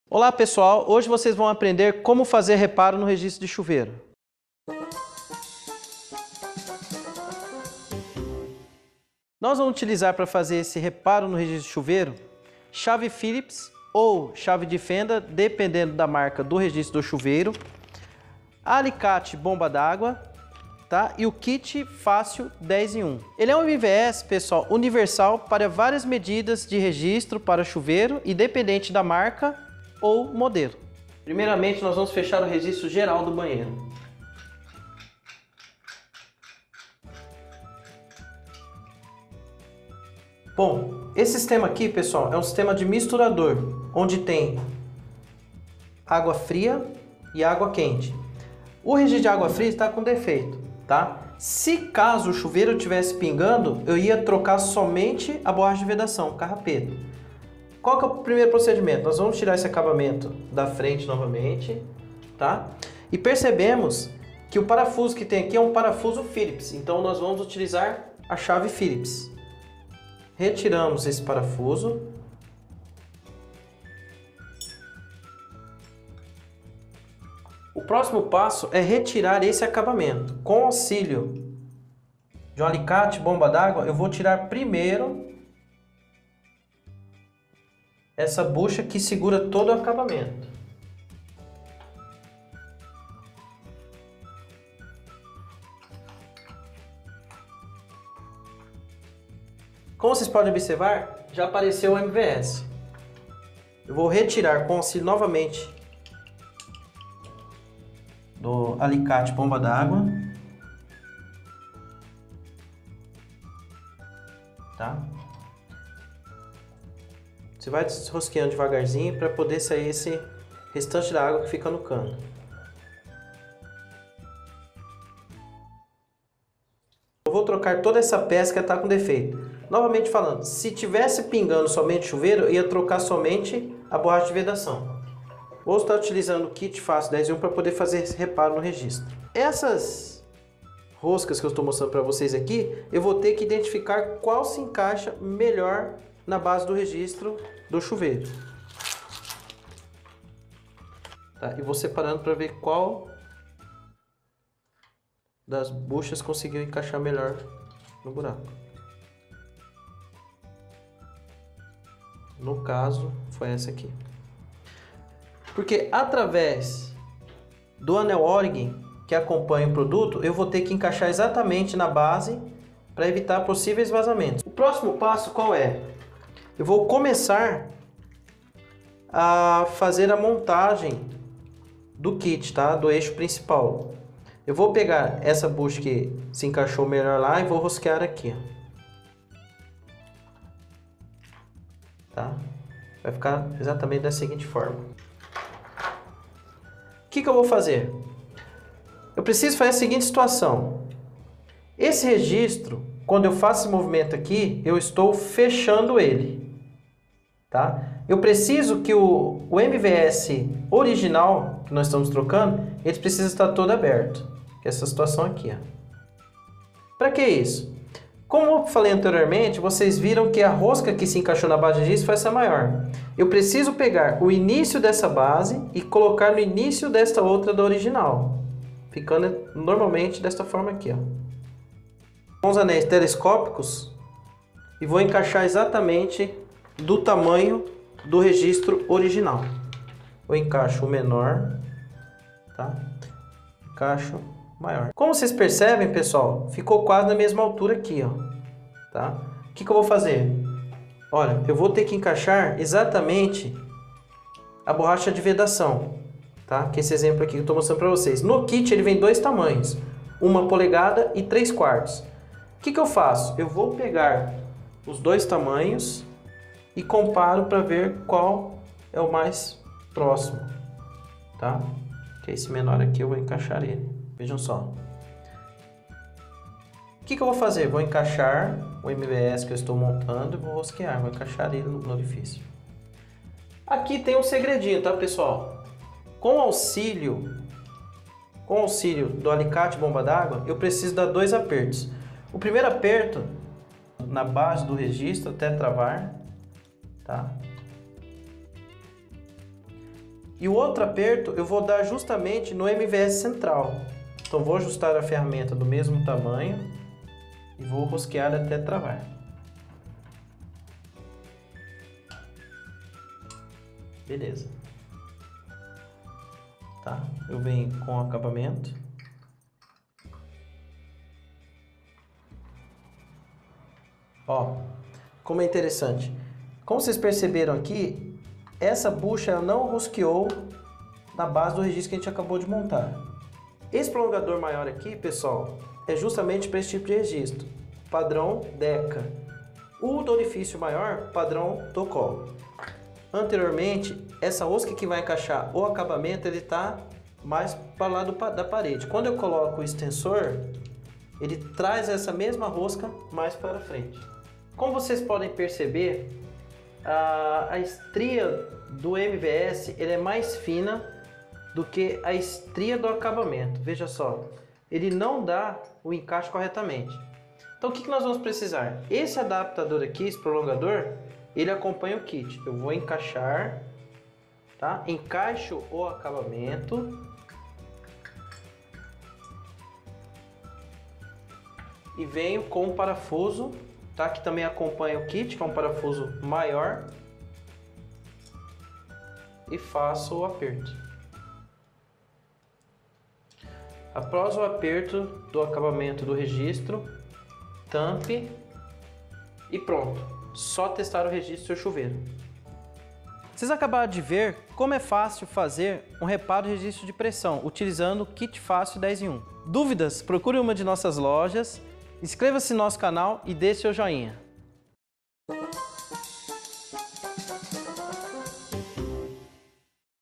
Como consertar o registro de seu chuveiro.
Compre pelo nosso site!

Faça você mesmo: Aprenda como acabar com o pinga-pinga do seu chuveiro.

Encontre a Loja Multicoisas mais próxima de você e os horários de funcionamento no